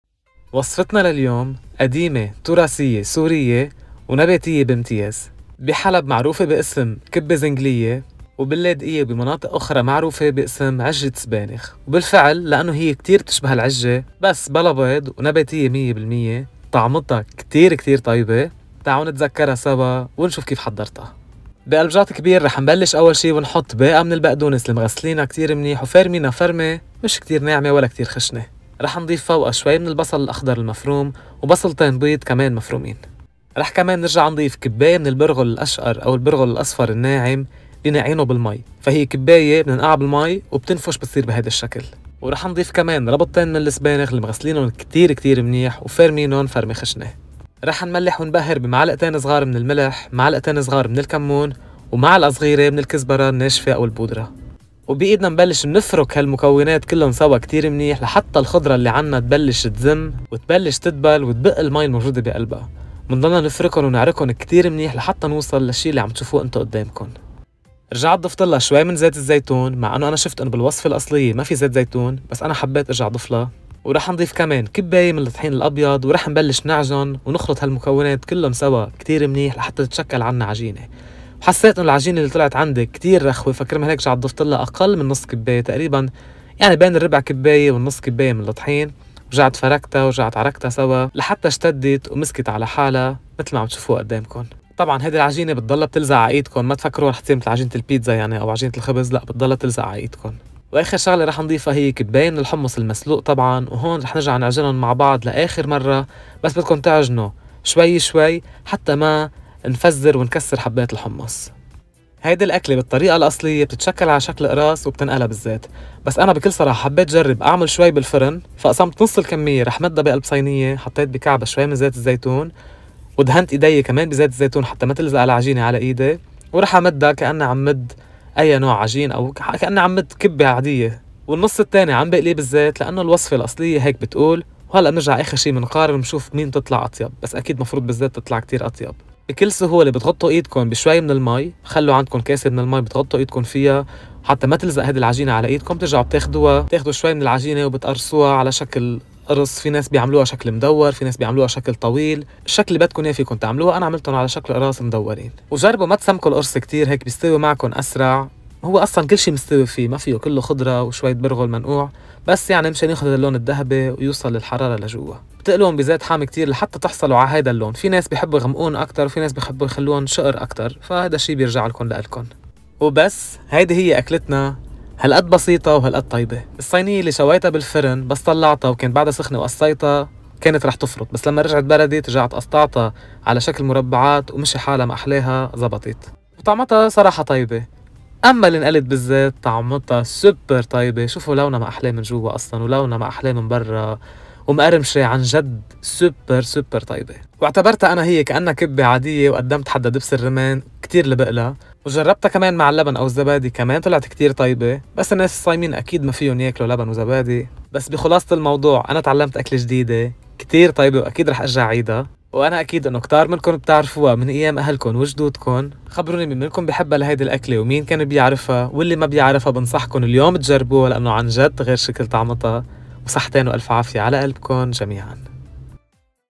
حسني رمضان | كبة زنكلية او عجة سبانخ؛ أكلة من التراث السوري بسيطة نباتية و كثير طيبة
كبة زنكلية او عجة سبانخ؛ أكلة من التراث السوري بسيطة نباتية و كثير طيبة🌱🤤😍.
المكونات:
١ باقة بقدونس
١ بصل أخضر
٢ بصل أبيض
١ كوب برغل أصفر ناعم (منقوع بالماء لمدة نص ساعة أقل شي)
٢ ربطة كبيرة سبانخ
٢ ملعقة صغيرة ملح
٢ ملعقة صغيرة كمون
١ ملعقة صغيرة كزبرة بودرة
٢ ملعقة كبيرة زيت زيتون
١ كوب الى ١ موب و نصف طحين أبيض
١ كوب حمص مسلوق
زيت للقلي.